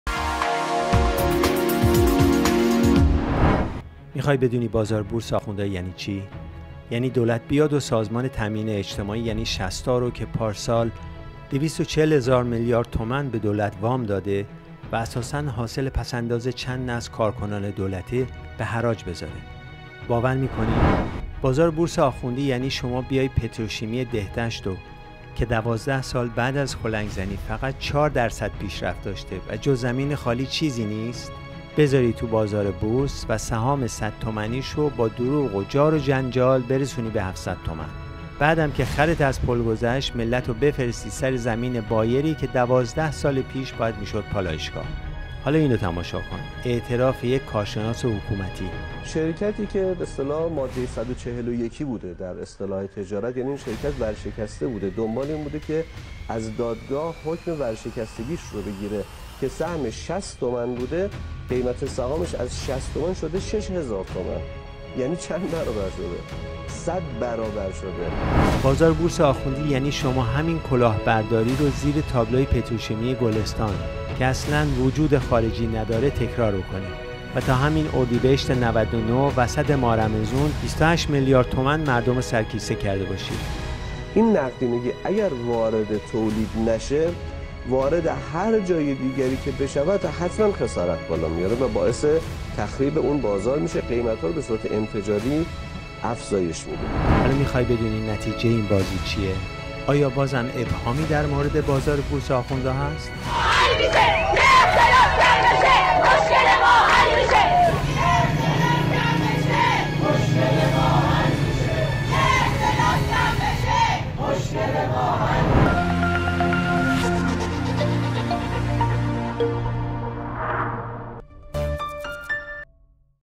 بازار بورس آخوندی یعنی چی؟
می‌خواهید بدانید بازار بورس آخوندها یعنی چه؟
یعنی دولت بیاید و سازمان تأمین اجتماعی (یعنی شستا) را که پارسال ۲۴۰هزار میلیارد تومان به دولت وام داده بود و اساساً حاصل پس‌انداز چند نسل کارکنان دولت است، به حراج بگذارد! باور می‌کنید؟
بازار بورس آخوندی یعنی شما بیایید پتروشیمی‌ دهدشت را که ۱۲سال بعد از کلنگ‌زنی فقط ۴درصد پیشرفت داشته و جز زمین خالی چیزی نیست، بگذارید توی بازار بورس و سهام صد تومانی آن را با دروغ و جار و جنجال برسانید به ۷۰۰تومان! بعد هم که خرتان از پل گذشت، ملت را بفرستید بر سر زمین بایری که ۱۲سال پیش باید پالایشگاه می‌شد!
حالا به این توجه کنید، اعتراف یک کارشناس حکومتی: «شرکتی که به‌اصطلاح ماده ۱۴۱ بوده در اصطلاح تجارت یعنی آن شرکت ورشکسته بوده دنبال این بوده که از دادگاه حکم ورشکستگی‌اش را بگیرد که سهمش ۶۰تومان بوده، قیمت سهامش از ۶۰تومان شده ۶هزار تومان یعنی چند برابر شده، ۱۰۰برابر شده».
بازار بورس آخوندی یعنی شما همین کلاهبرداری را زیر تابلوی پتروشیمی گلستان که اصلاً وجود خارجی ندارد تکرار کنید و تا همین اردیبهشت۹۹ در وسط ماه رمضان ۲۸میلیارد تومان مردم را سرکیسه کرده باشید!
حالا می‌خواید بدانید نتیجه این بازی چیست؟
«این نقدینگی اگر وارد تولید نشود وارد هر جای دیگری که بشود حتماً خسارت بالا می‌آورد و باعث تخریب آن بازار می‌شود قیمتها را به‌صورت انفجاری افزایش می‌دهد».
آیا باز هم ابهامی در مورد بازار بورس آخوندها وجود دارد؟!